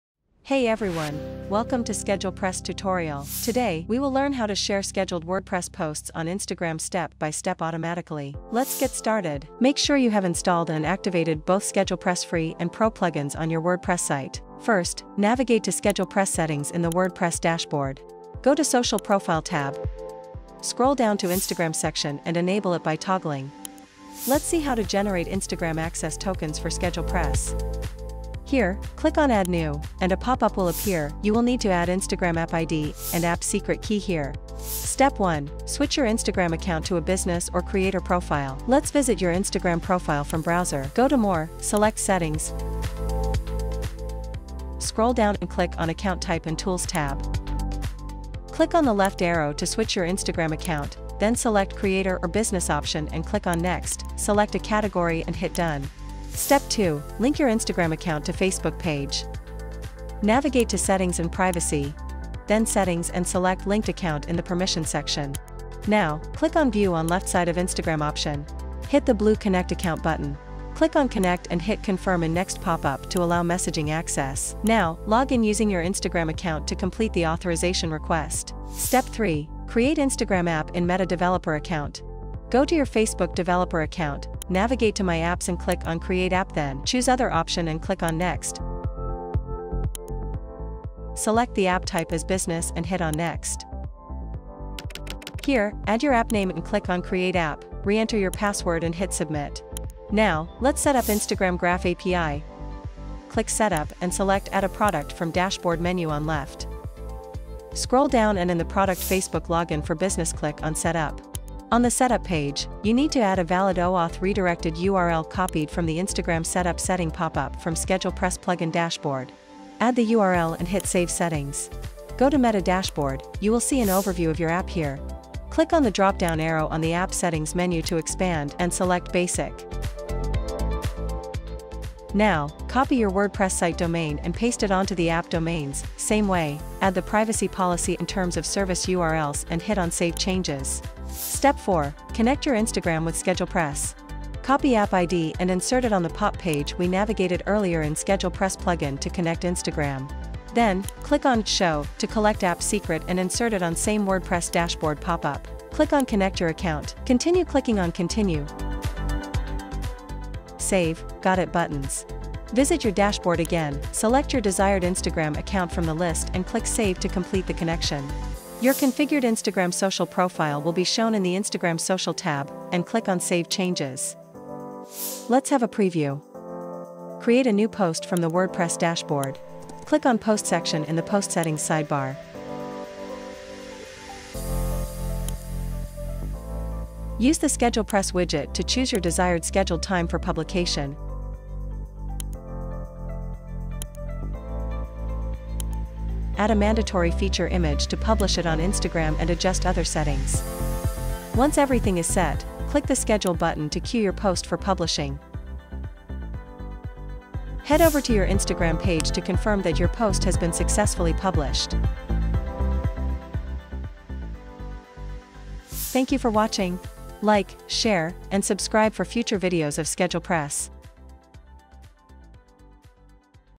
How To Automatically Share Scheduled WordPress Posts On Instagram
Want to keep your Instagram feed fresh but need more time for manual posting? This video is for you! We'll show you how to set up SchedulePress to automatically share your scheduled WordPress posts to Instagram, saving you tons of time!

This tutorial takes you through the entire process, from setting up SchedulePress for Instagram to connecting your accounts and scheduling posts. Follow our step-by-step guide and prepare to take your social media marketing to the next level!